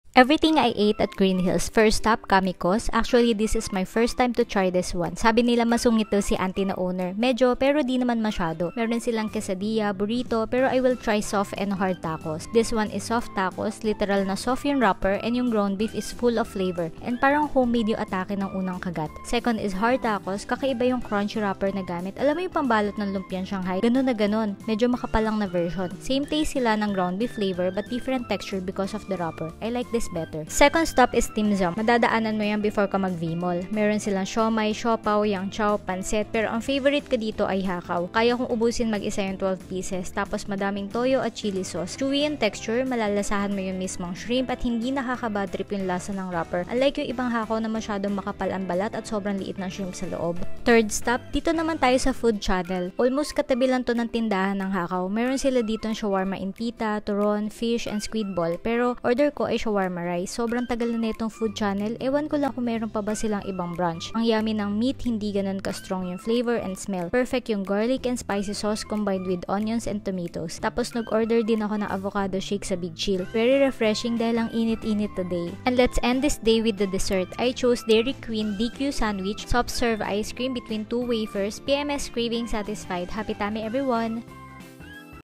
Everything I ate at Greenhills | Hakaw Tacos Shawarma Rice | FOODTRIP sa San Juan
I hope you learned something valuable today =)